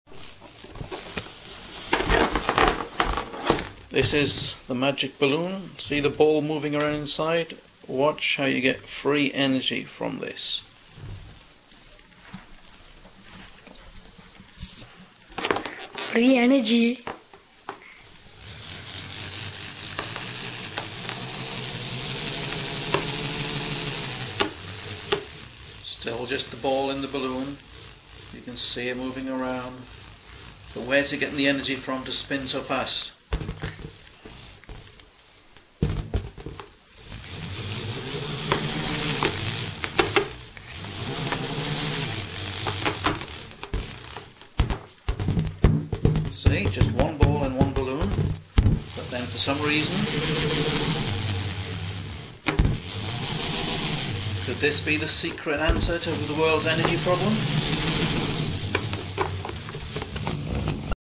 Could this be the answer to the world energy problem
For year people have attempted to turn reciprocal motion into rotary motion efficiently!

Now could this me the solution everyone has been trying to achieve!

How does the the ball in the balloon reach such high speeds?